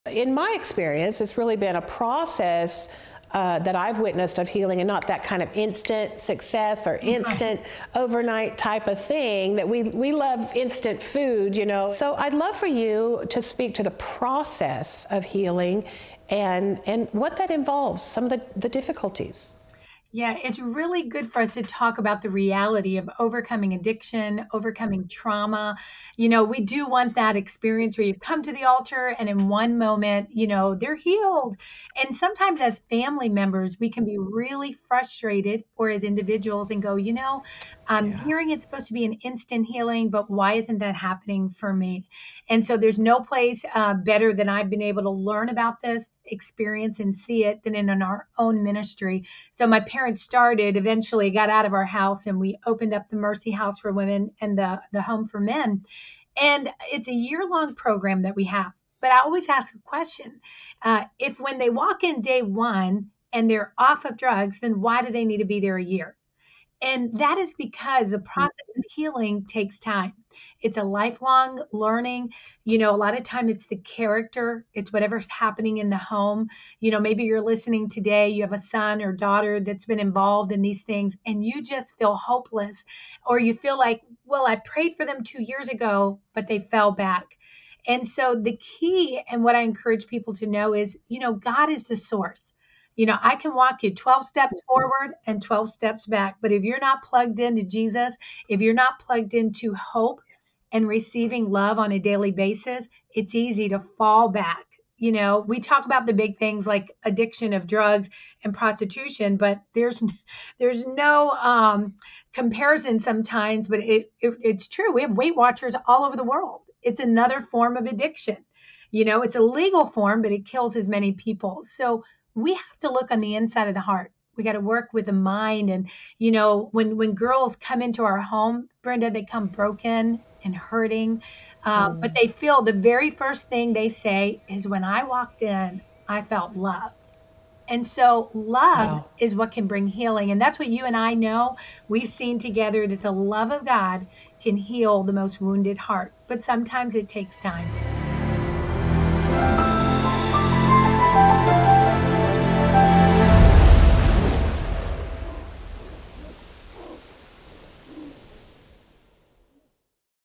What Does Healing Look Like?
Do you feel like you've been praying for healing and nothing's come from it, or like you keep falling backwards? @Rosalindarivera1breaks down the truth of what it takes to break free from our traumas & addictions.